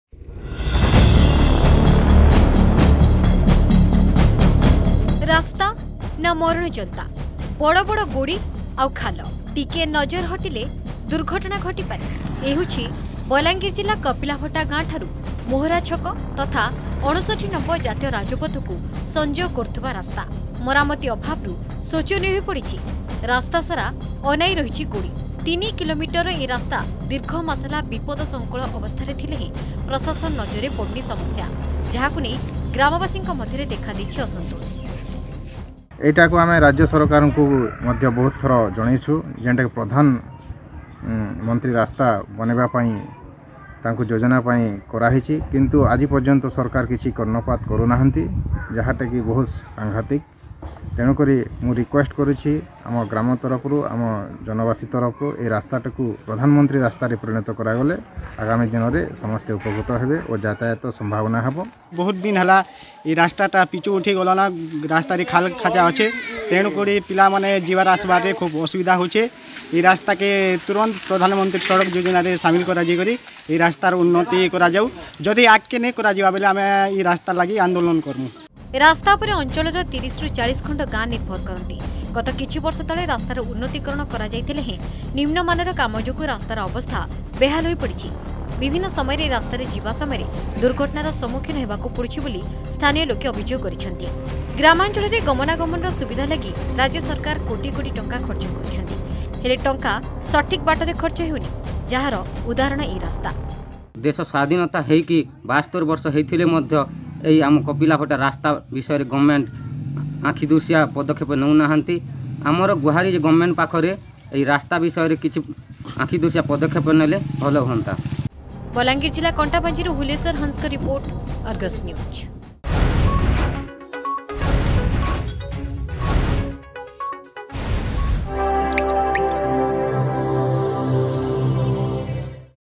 Road or Deathtrap; No sign of development in the rural roads in kapilabhata, Balangir
Road from Kapilavata village in Balangir district to Mohra Chowk and National Highway 59 turned into death trap due to the negligence of the Odisha Government.